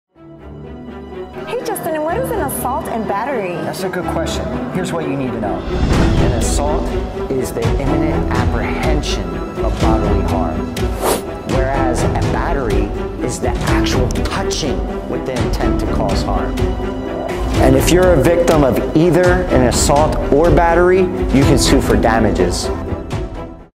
💥😫🤛Assault & Battery: What’s The Difference? | Lawyer Explains | Will Smith Oscars
You NEED to know these differences!
Call or Text 24/7: (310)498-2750